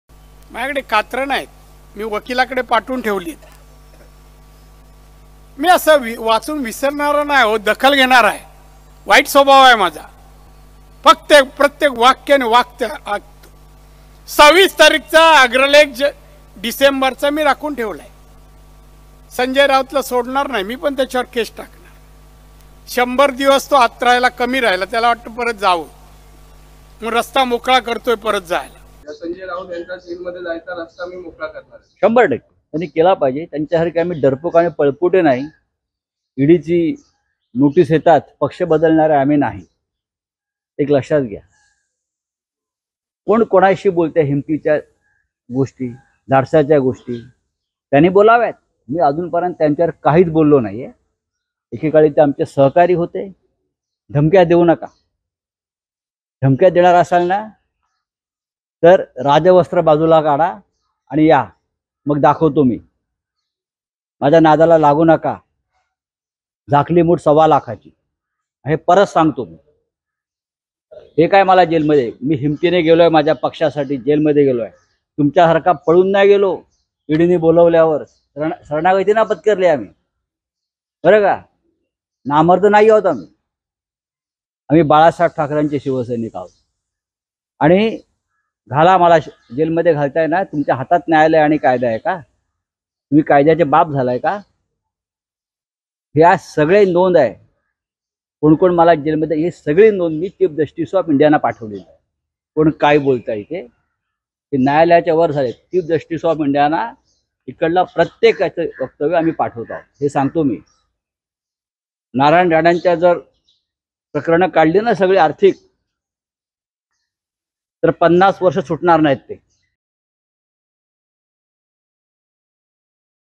Narayan Rane Vs Sanjay Raut : नारायण राणे-संजय राऊत भिडले... | LetsUpp Marathi
संजय राऊत यांना आपण पुन्हा जेलवारी घडवणार, असल्याचा घणाघात केंद्रीय मंत्री नारायण राणे यांनी केला. .यानंतर राऊतांनी देखील तिखट शब्दांमध्ये 'माझ्या नादाला लागू नका. झाकली मुठ सव्वा लाखाची', असे म्हणत पलटवार केला.